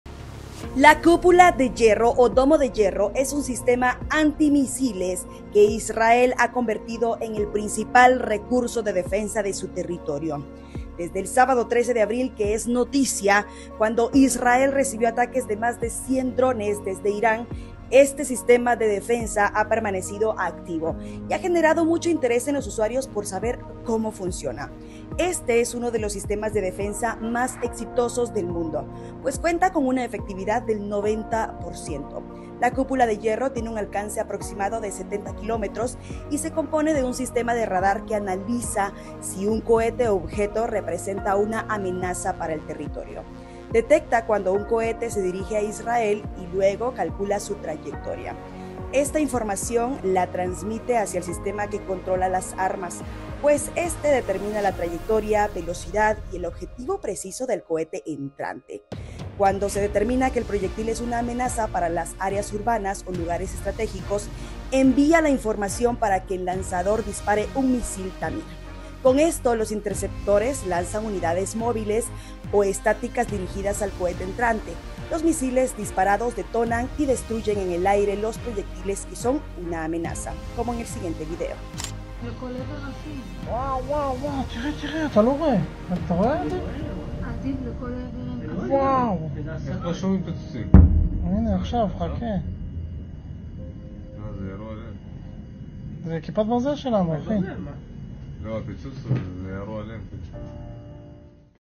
Cúpula de Hierro de Israel: así funciona el sistema antimisiles
La Cúpula de Hierro o Domo de Hierro es un sistema antimisiles que Israel ha convertido en el principal recurso de defensa de su territorio.
Desde el sábado 13 de abril, cuando Israel recibió ataques de más de cien drones desde Irán, este sistema de defensa ha permanecido activo y ha generado mucho interés en los usuarios por saber cómo funciona.